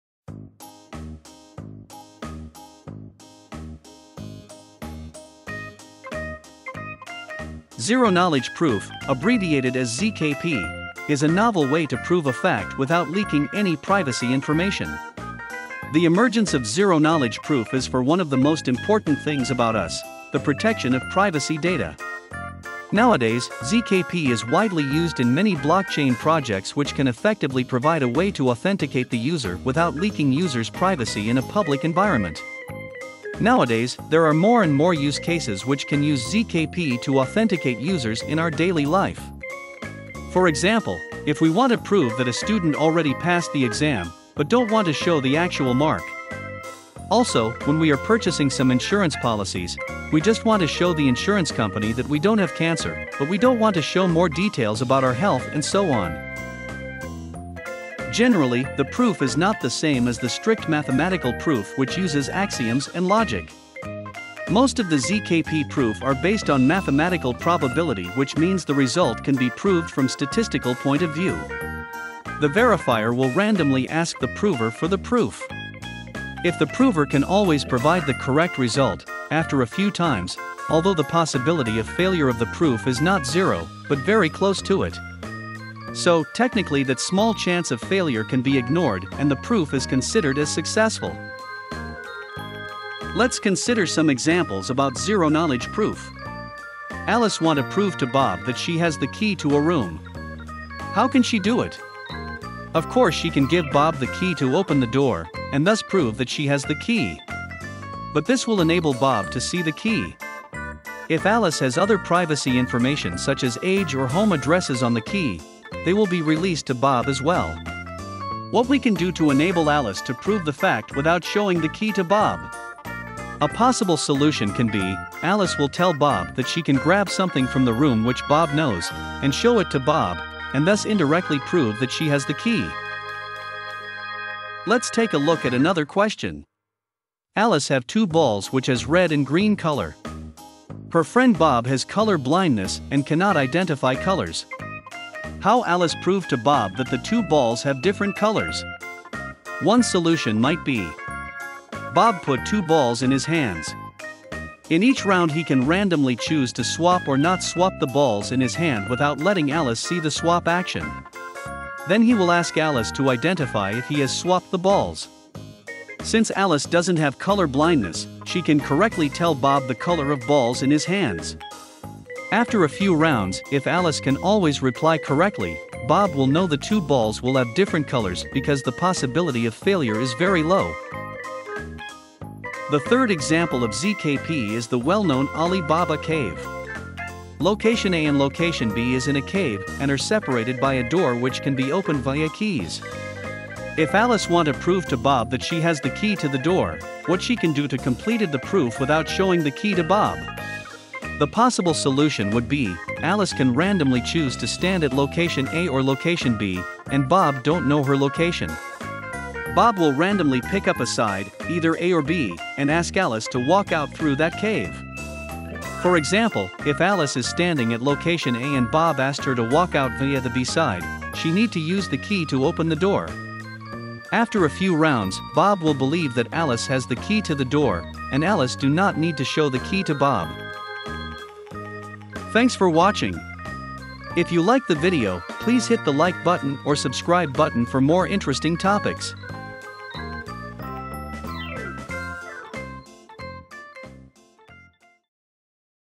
Zero Knowledge Proof Explained By Examples
Zero-Knowledge Proof, abbreviated as ZKP, is a novel way to prove a fact without leaking any privacy information. The emergence of Zero-Knowledge Proof is for one of the most important things about us, the protection of privacy data. Nowadays, ZKP is widely used in many block-chain projects which can effectively provide a way to authenticate the user without leaking user's privacy in a public environment. Nowadays, there are more and more use cases which can use ZKP to authenticate users in our daily life. For example, if we want to prove that a student already passed the exam but don't want to show the actual mark for the exam. Also, when we are purchasing some insurance policies, we just want to show the insurance company that we don't have cancer, but we don't want to show more details about our health, and so on.

Generally, the proof is not the same as the strict mathematical proof which uses axioms and logic. Most of the ZKP proof are based on mathematical Probability which means the result can proved from statistical point of view. The verifier will randomly ask the prover for the proof. If the prover can always provide the correct result, after a few times, although the possibility of failure of the proof is not zero, but very close to it. So, technically that small chance of failure can be ignored and the proof is considered as successful.

Let's consider some examples about Zero-Knowledge Proof. Alice want to prove to Bob that she has the key to a room. How can she do it? Of course she can give Bob the key to open the door, and thus prove that she has the key. But this will enable Bob to see the key. If Alice has other privacy information such as age or home addresses on the key, they will be released to Bob as well. What we can do to enable Alice to prove the fact without showing the key to Bob? A possible solution can be: Alice will tell Bob that she can grab something from the room which Bob knows, and show it to Bob, and thus indirectly prove that she has the key.

Let's take a look at another question. Alice have two balls which has red and green color. Her friend Bob has color blindness and cannot identify colors. How Alice prove to Bob that the two balls have different colors? One solution might be. Bob put two balls in his hands. In each round he can randomly choose to swap or not swap the balls in his hand without letting Alice see the swap action. Then he will ask Alice to identify if he has swapped the balls. Since Alice doesn't have color blindness, she can correctly the color of balls in Bob's hands. After a few rounds, If Alice can always reply correctly, Bob will know the two balls will have different colors because the possibility of failure is very low.

The third example of ZKP is the well-known Ali Baba cave. Location A and Location B is in a cave and are separated by a door which can be opened via keys. If Alice want to prove to Bob that she has the key to the door, what she can do to completed the proof without showing the key to Bob? The possible solution would be: Alice can randomly choose to stand at location A or location B, and Bob don't know her location. Bob will randomly pick up a side, either A or B, and ask Alice to walk out through that cave. For example, if Alice is standing at location A and Bob asked her to walk out via the B side, she need to use the key to open the door. After a few rounds, Bob will believe that Alice has the key to the door, and Alice do not need to show the key to Bob.